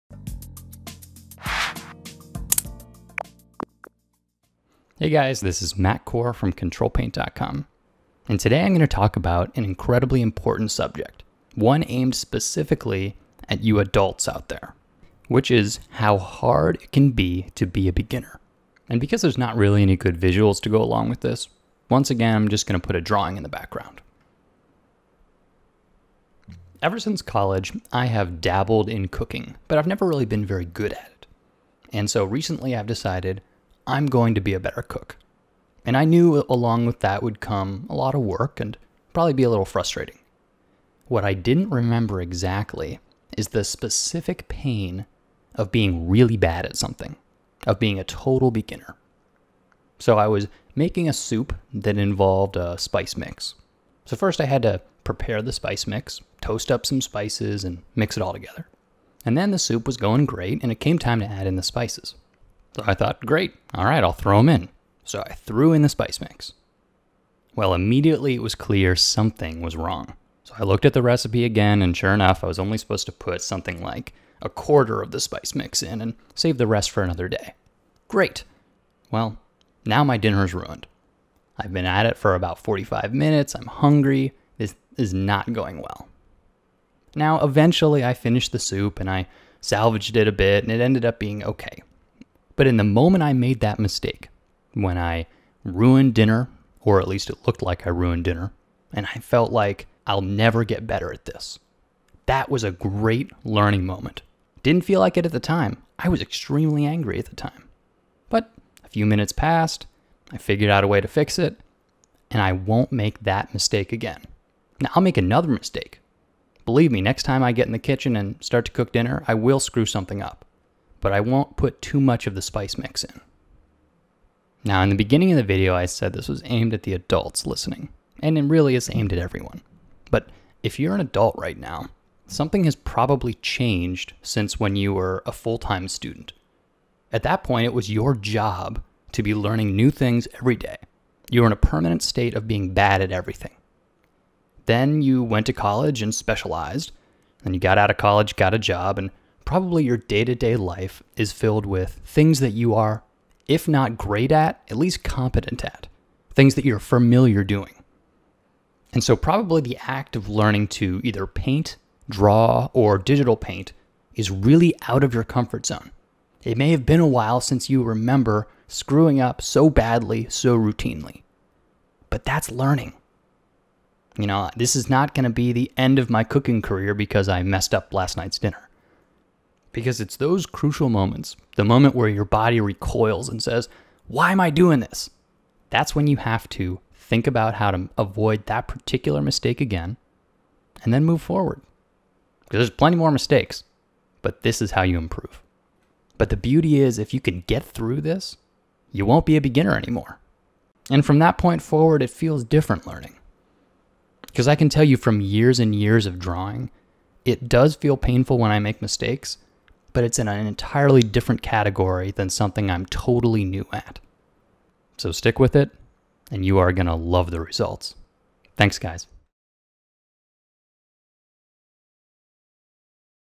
Being a Beginner | Digital Art
It's hard to be a beginner.  This video aims to give a bit of perspective and encouragement.  Additionally, it will help you avoid some mistakes in the kitchen.